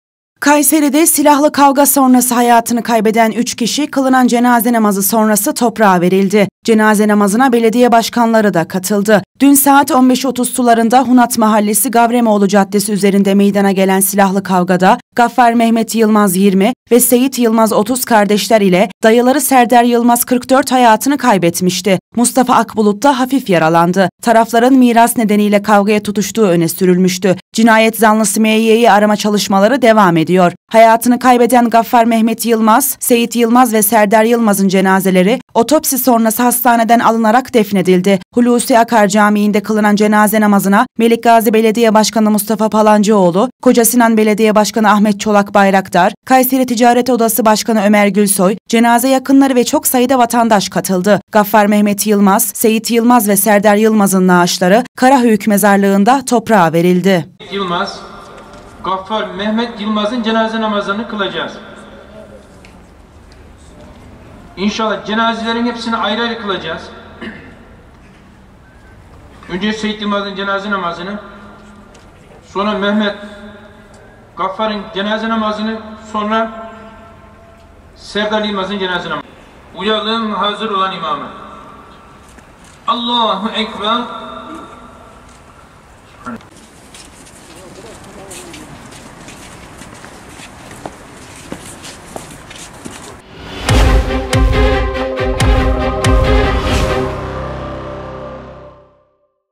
Miras Kavgasında Hayatını Kaybeden 3 Kişi Defnedildi
KAYSERİ, (BGC)- Kayseri'de silahlı kavga sonrası hayatını kaybeden 3 kişi, kılınan cenaze namazı sonrası toprağa verildi. Cenaze namazına belediye başkanları da katıldı.

Dün saat 15.30 sularında Hunat Mahallesi Gavremoğlu Caddesi üzerinde meydana gelen silahlı kavgada, Gaffar Mehmet Yılmaz (20) ve Seyit Yılmaz (30) kardeşler ile dayıları Serdar Yılmaz (44) hayatanı kaybetmişti. Mustafa Akbulut da hafif yaralandı. Tarafların miras nedeniyle kavgaya tutuştuğu öne sürülmüştü. Cinayet zanlısı M.Y.’yi arama çalışmaları devam ediyor.

Hayatını kaybeden Gaffar Mehmet Yılmaz, Seyit Yılmaz ve Serdar Yılmaz'ın cenazeleri otopsi sonrası hastaneden alınarak defnedildi. Hulusi Akar Camii'nde kılınan cenaze namazına Melikgazi Belediye Başkanı Mustafa Palancıoğlu, Kocasinan Belediye Başkanı Ahmet Çolakbayrakdar, Kayseri Ticaret Odası (KTO) Başkanı Ömer Gülsoy, cenaze yakınları ve çok sayıda vatandaş katıldı. Gaffar Mehmet Yılmaz, Seyit Yılmaz ve Serdar Yılmaz'ın naaşları, Karahöyük Mezarlığı'nda toprağa verildi.